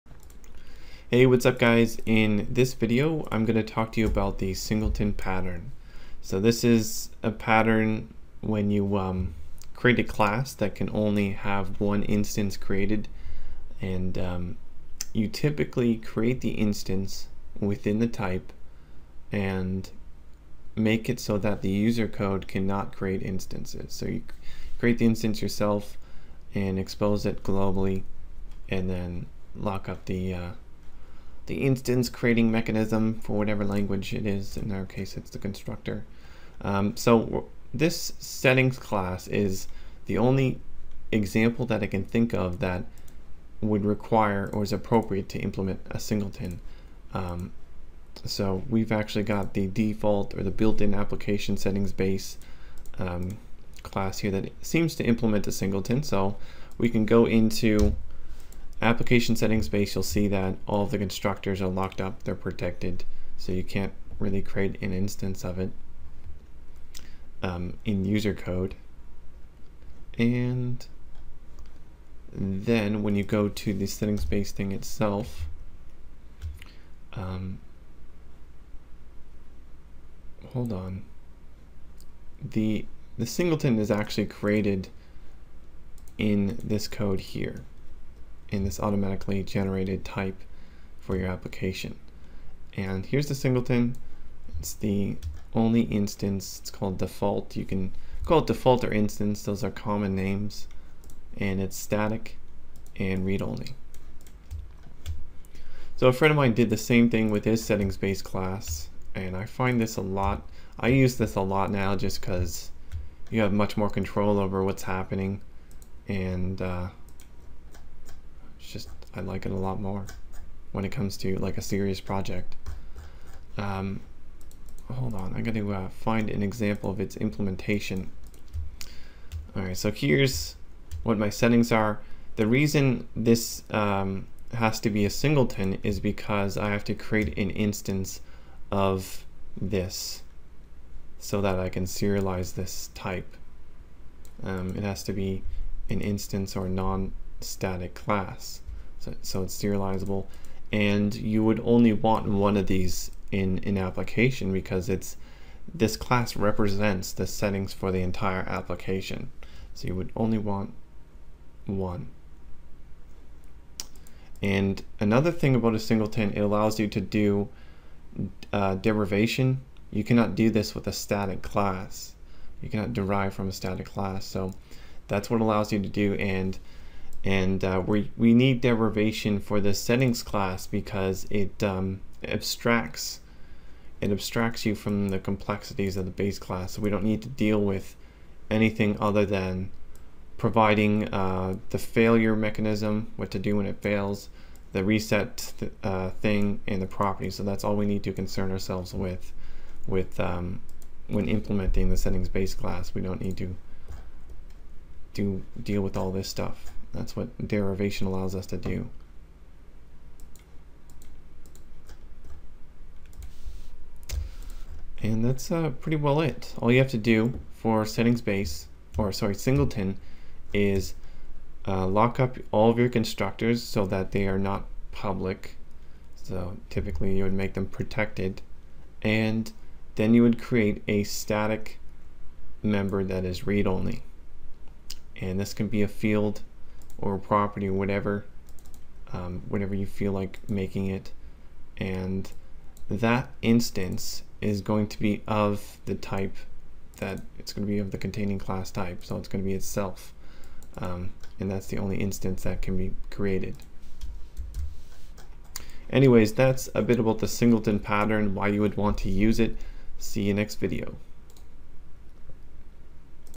When to consider a singleton
You basically want a singleton when you are sure it is never appropriate to create more than one instance of the class per assembly, yet you require an instance for one reason or another (derivation, easy serialization, properly representing a model). And singletons can ease implementation as only the type needs to be defined and no instances need to be created manually.